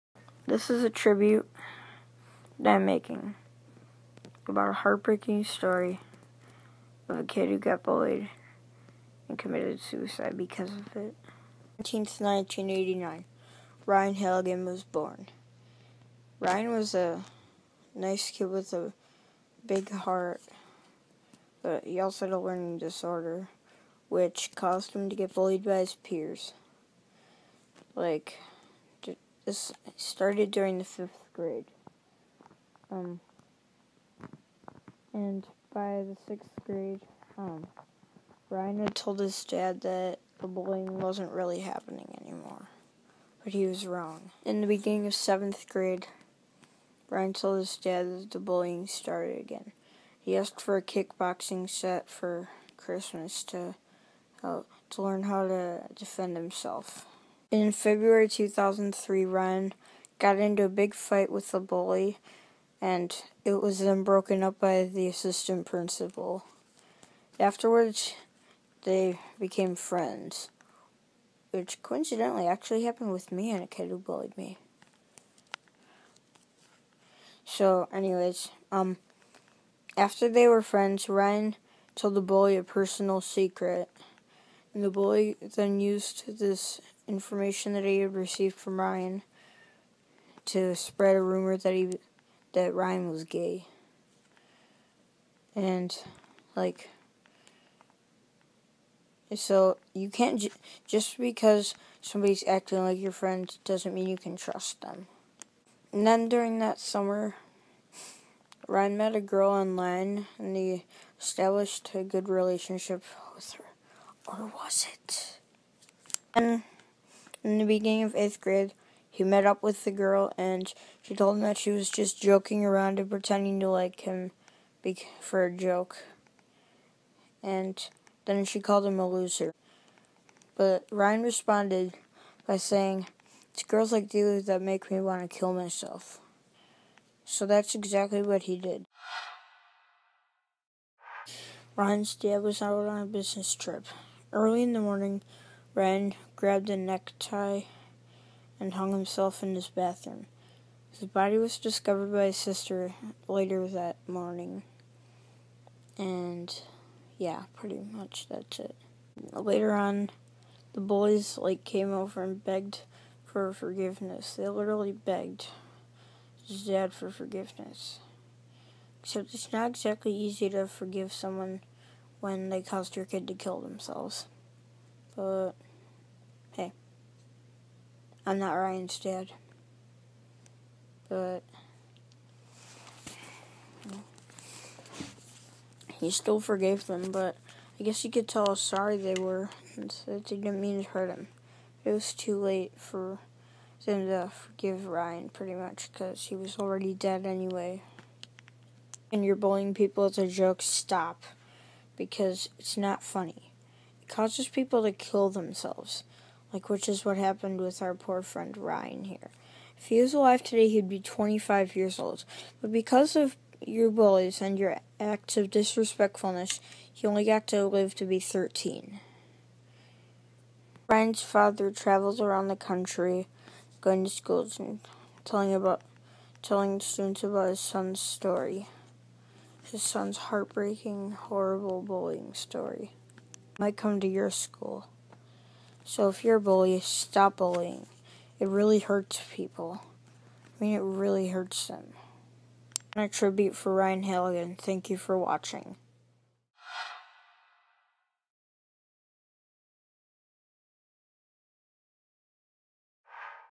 A tribute to Ryan Halligan
Ryan Halligan committed suicide from bullying 12 years ago today. End bullying!